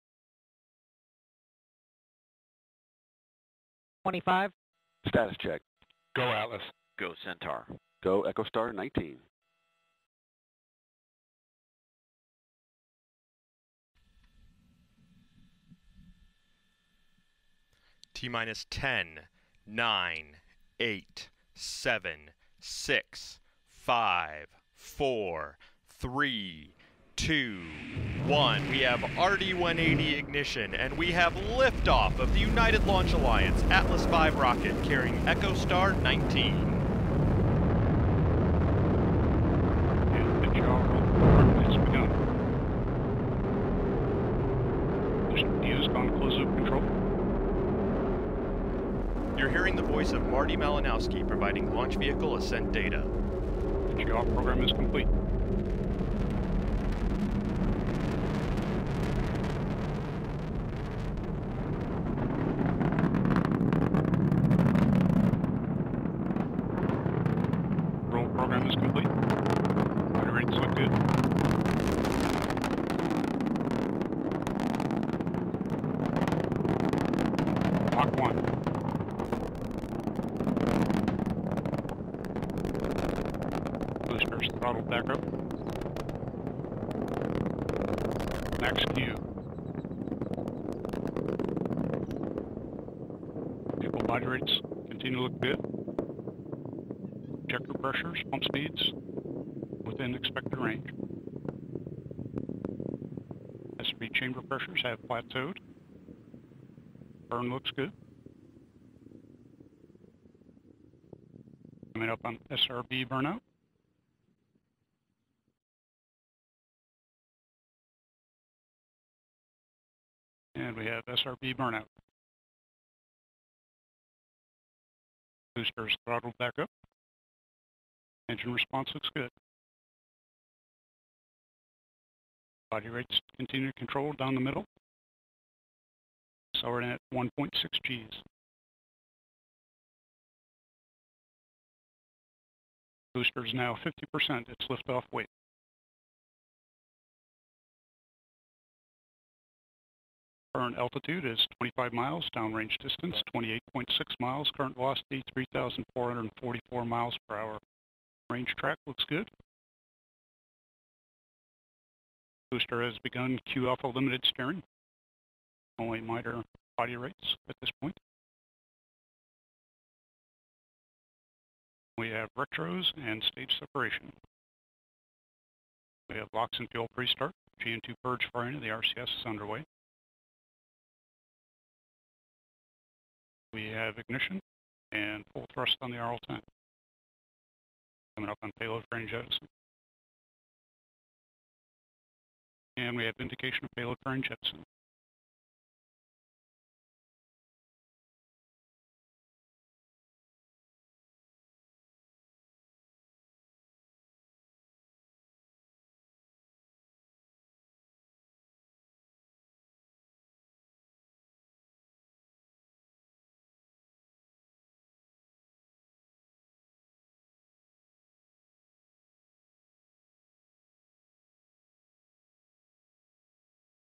Atlas V launches EchoStar XIX
A United Launch Alliance Atlas V 431 configuration rocket launched the EchoStar XIX mission from Space Launch Complex (SLC)-41 at Cape Canaveral Air Force Station, Florida, on 18 December 2016, at 19:13 UTC (14:13 EST). Echostar 19 (Jupiter-2) will be the world’s highest capacity broadband satellite in orbit. EchoStar XIX marks the 68th launch of the Atlas V rocket, the third in the 431 configuration and ULA’s 115th mission.

Credit:
United Launch Alliance